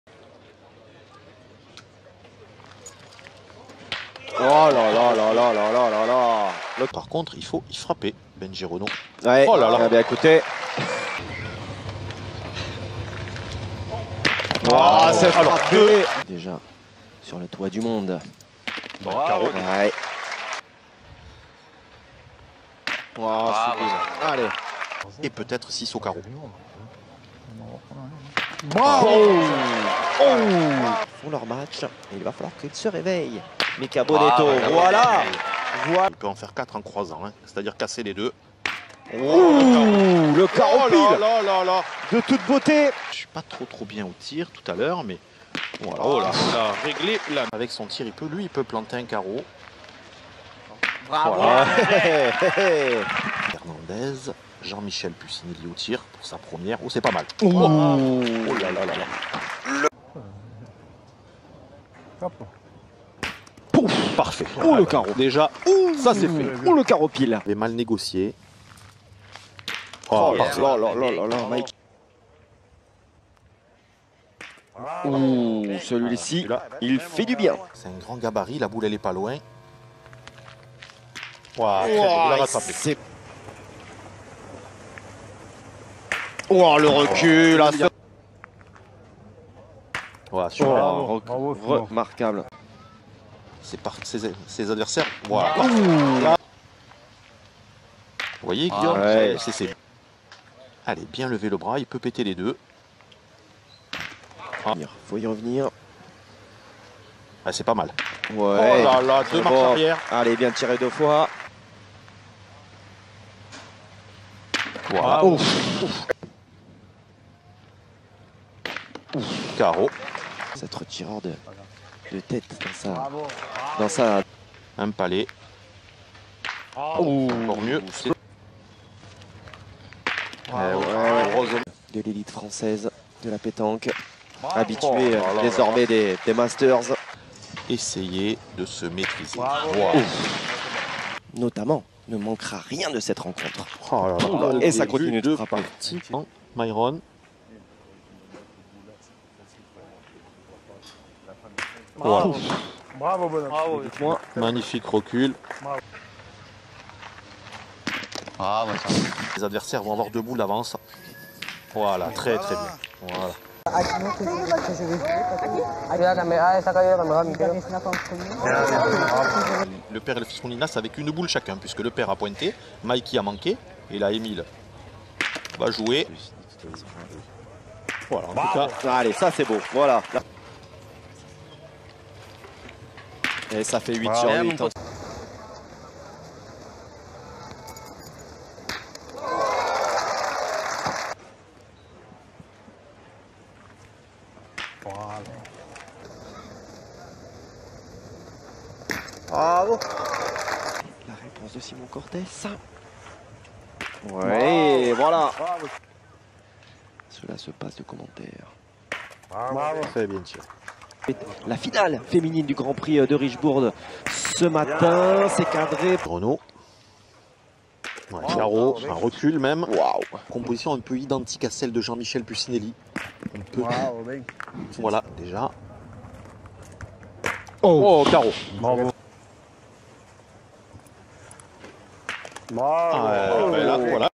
Pluie de carreaux au mondial la marseillaise à pétanque 2020 💣💣
J'ai fait une sélection des plus beaux carreaux de la marseillaise.
Bon visionnage.

Source vidéo: france3